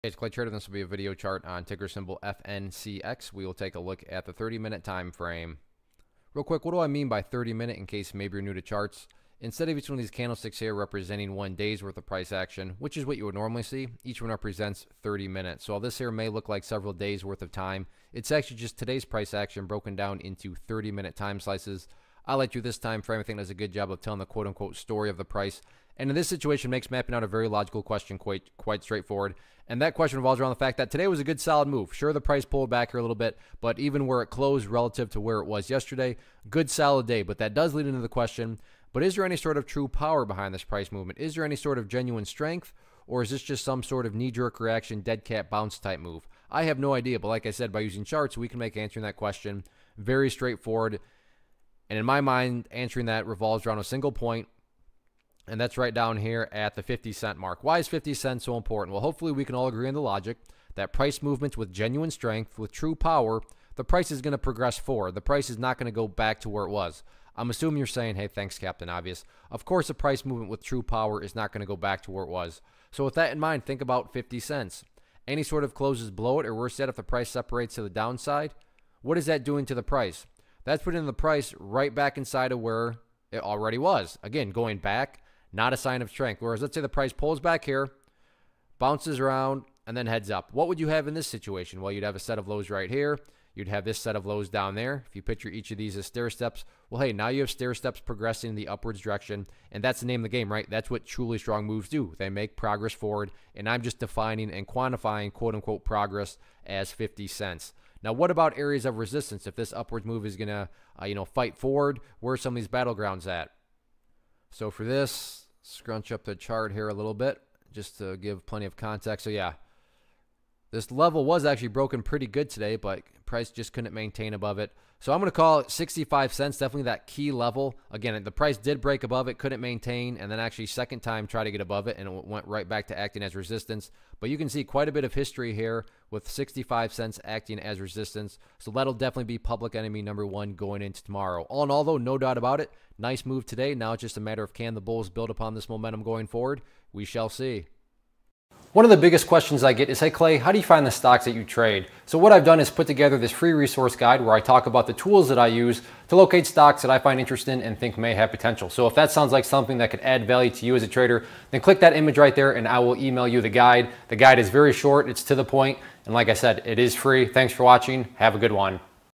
DraftDay Fantasy Sports - FNCX Stock Chart Technical Analysis for 04-12-17
Learn how to read stock charts and identify technical patterns as ClayTrader does a quick stock chart review on DraftDay Fantasy Sports (FNCX).